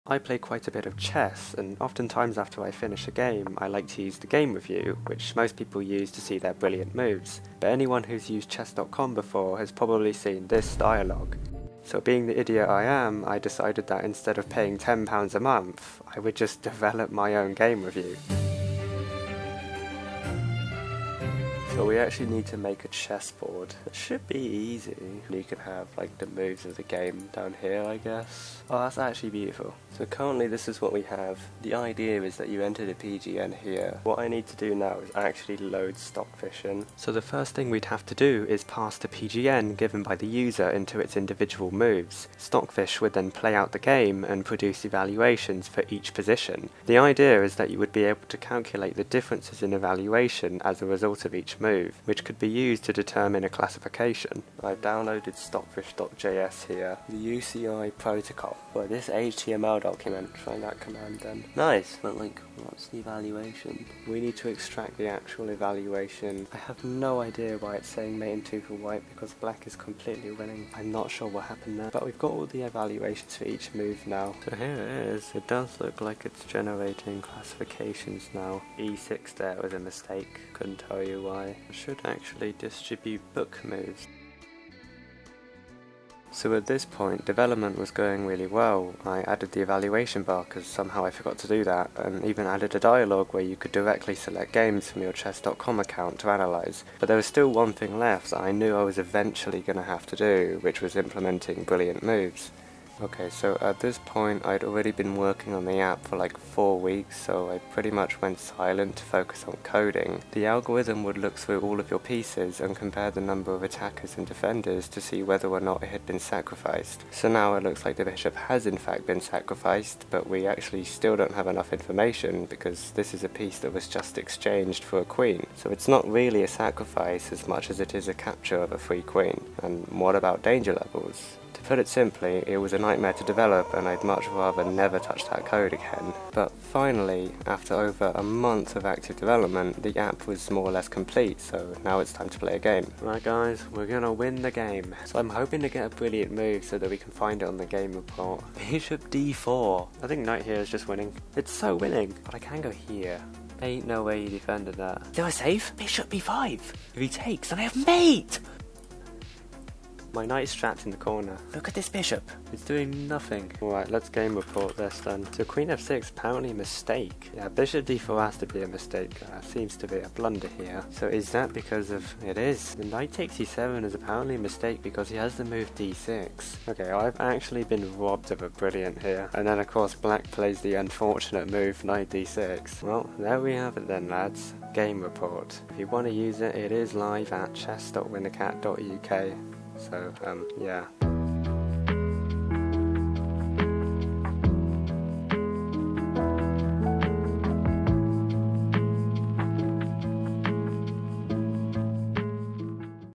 I coded a FREE Chess Game Review website.
How I coded a website to get Chess Game Reviews for free.

Improve your Chess by analysing your games for your mistakes and brilliant moves. This project took way longer than I was expecting it to, and I'm really glad to finally have it finished. I learnt a massive amount while building this app and I hope you'll find it useful!

Thanks :)